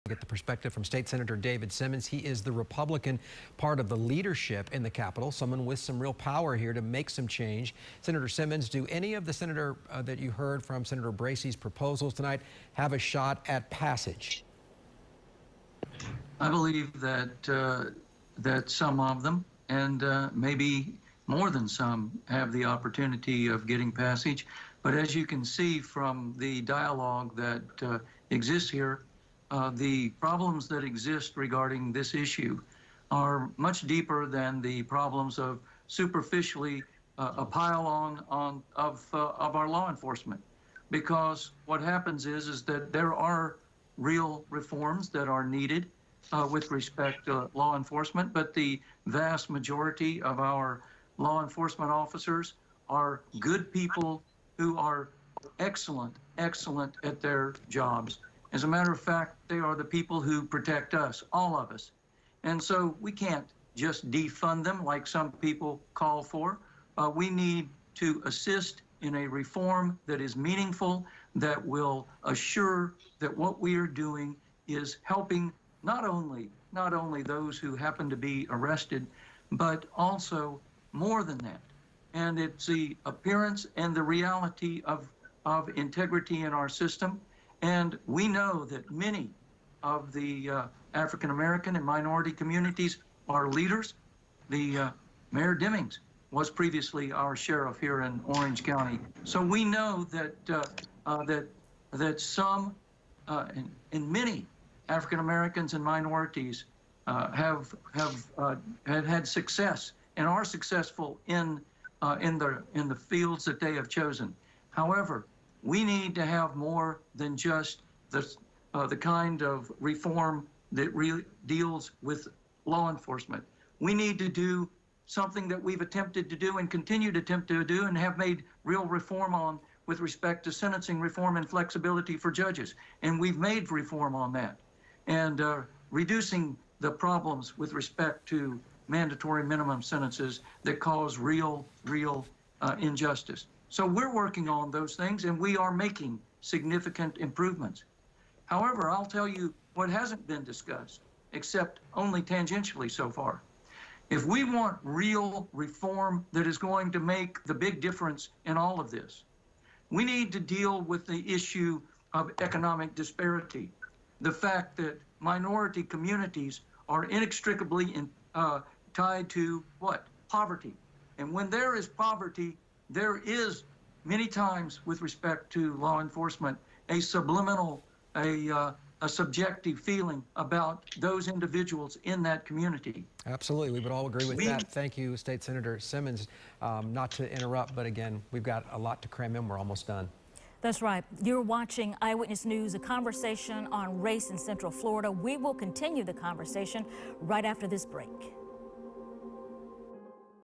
VIDEO: Reforms needed, but majority of law enforcement are good people, FL senator says
Florida State Sen. David Simmons, a Republican who represents District 9, joined the conversation during WFTV's live town hall on race Tuesday night.